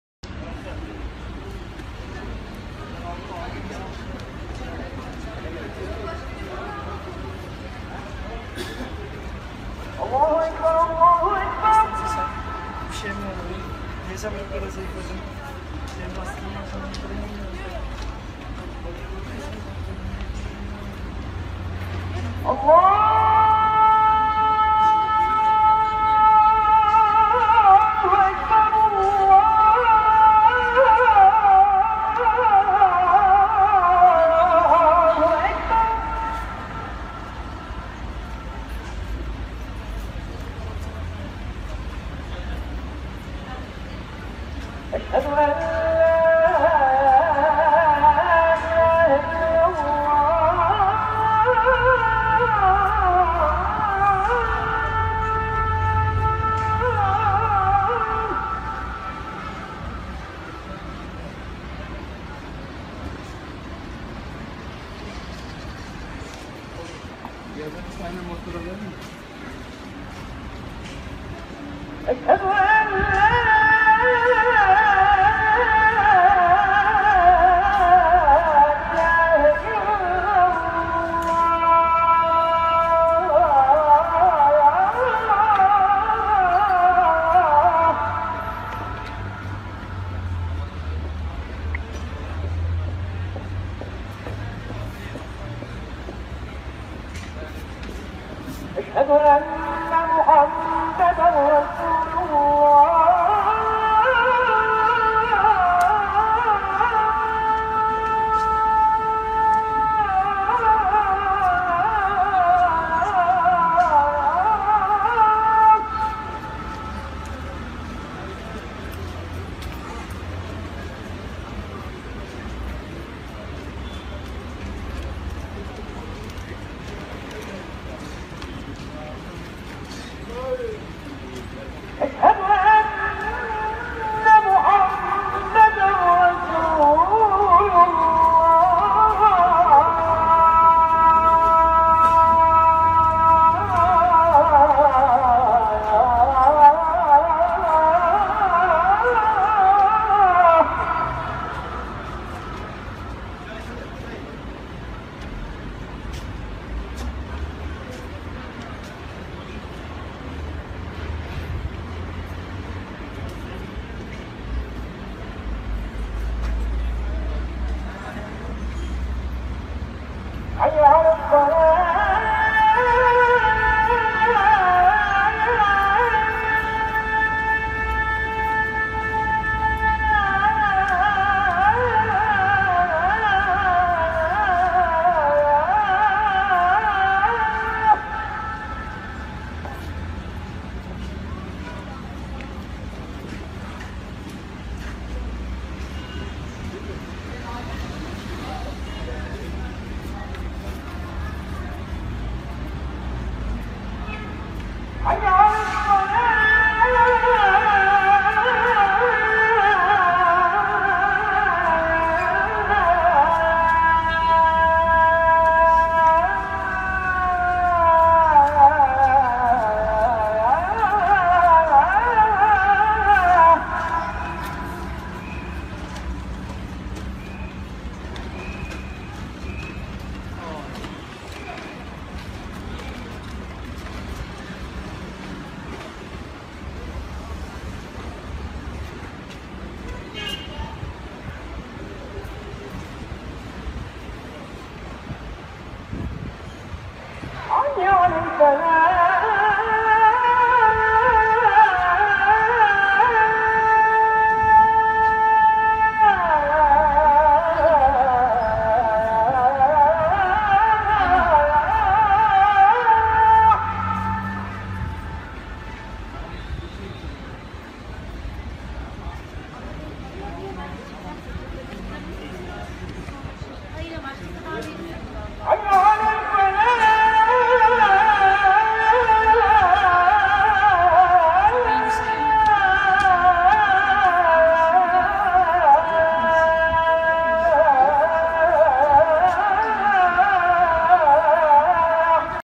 Alpcan ÇELİK Uşşak Ezan Bursa Ulu Cami
Dünya Kur'an okuma birincisi Alpcan ÇELİK'in Bursa Ulu Camii'de okumuş olduğu Uşşak makamındaki harika ezan.